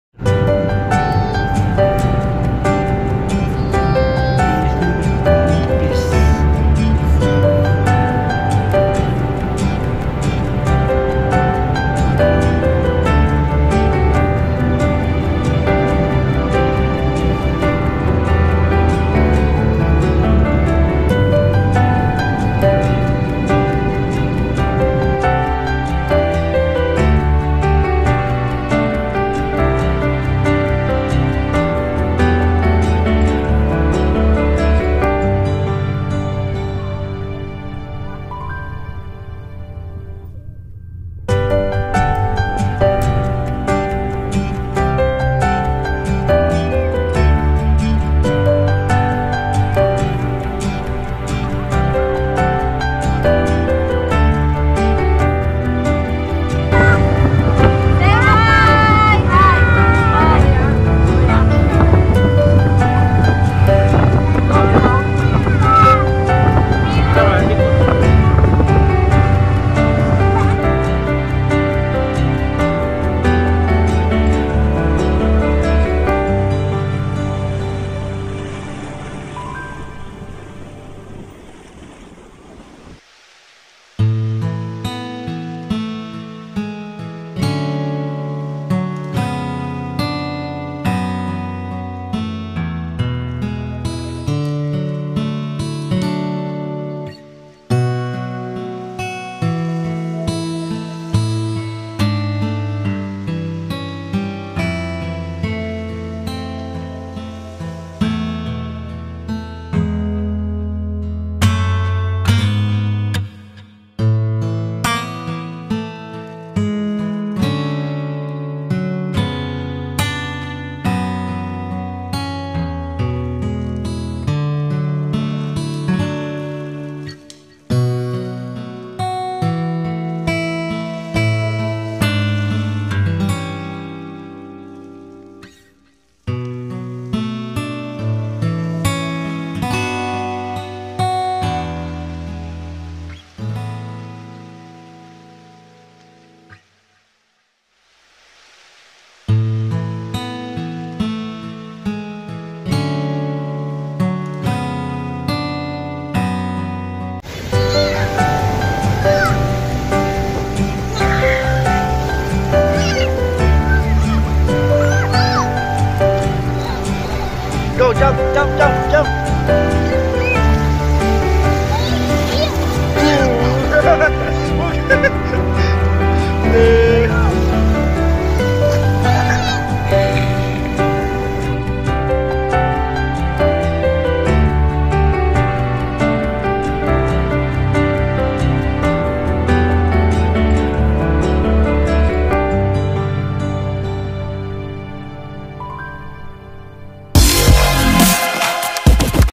May Mensahe Ang Alon Anak
bawat alon katumabas ng problema o pag subok,,
pag natumba ka padala ka lang pero pilitin mung makabangon,,
alam kong mahina ka nak pero matapang kang humaharap sa bawat alon,,